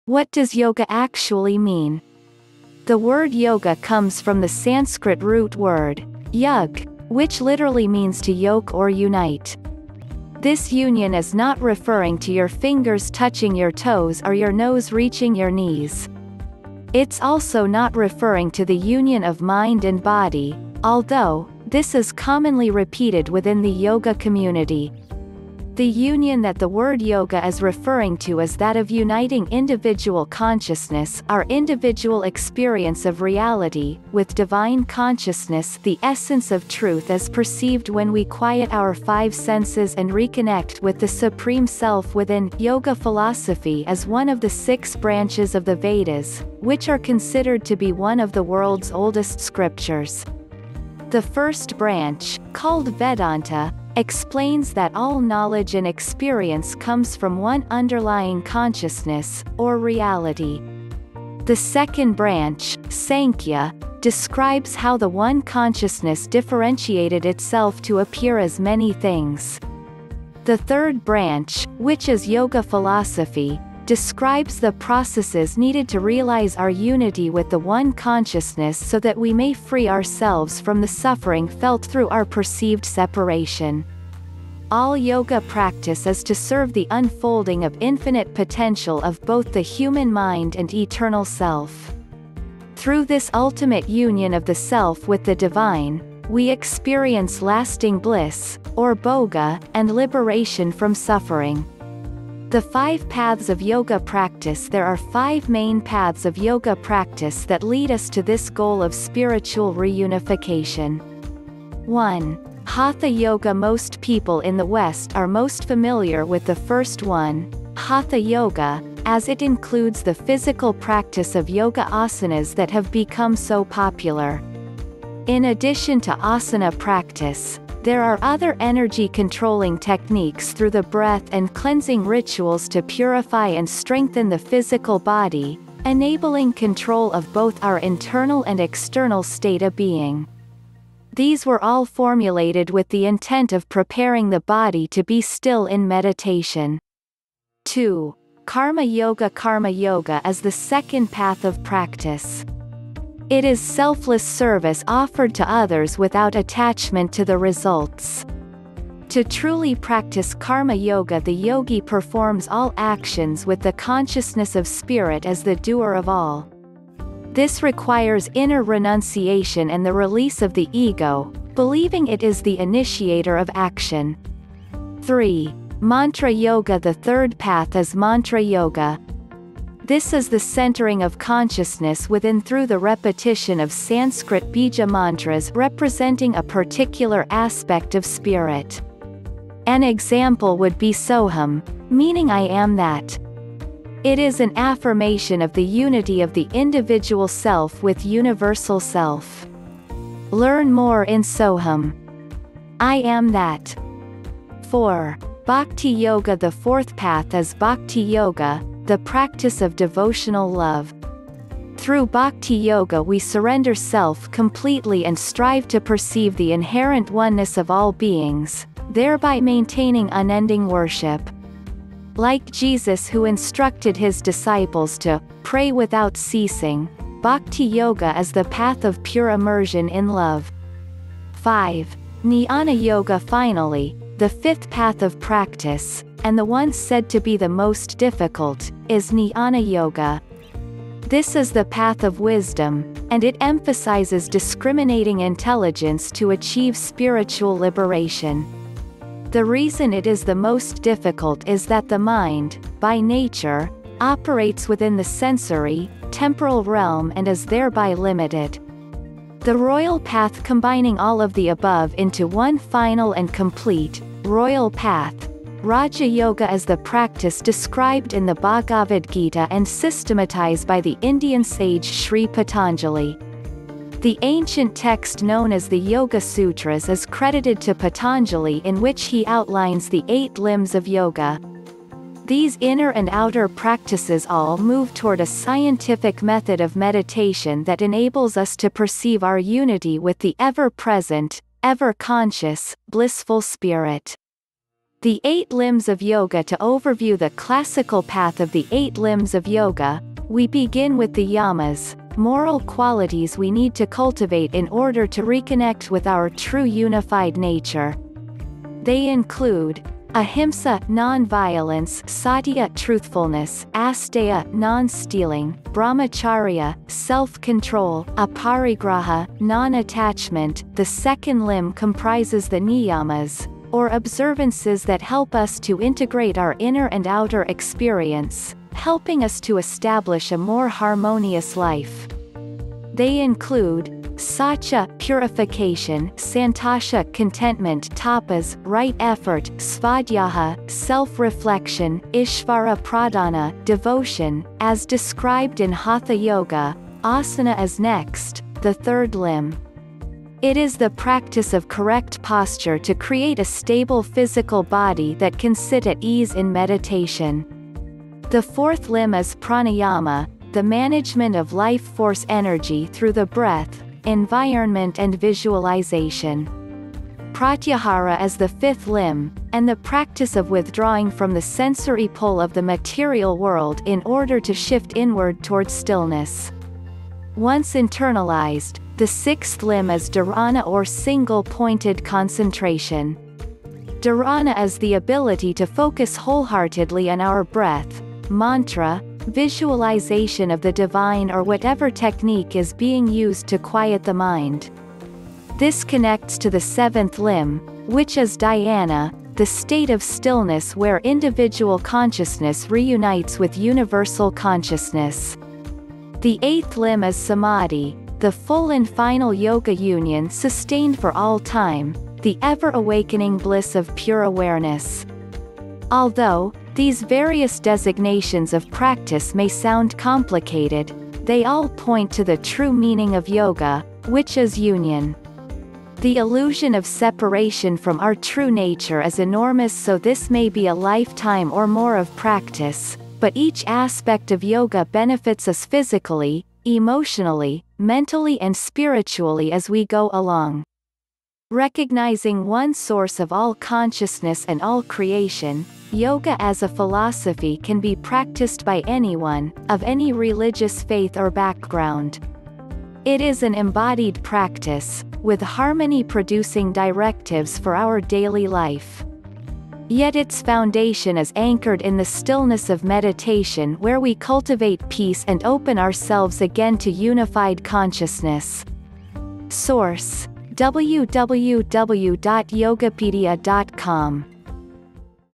What is the true meaning of yoga.
what is the true meaning of yoga.The word yoga comes from the Sanskrit root word “yug,” which literally means to yoke or unite.
This union is not referring to your fingers touching your toes or your nose reaching your knees. It's also not referring to the union of mind and body, although, this is commonly repeated within the yoga community.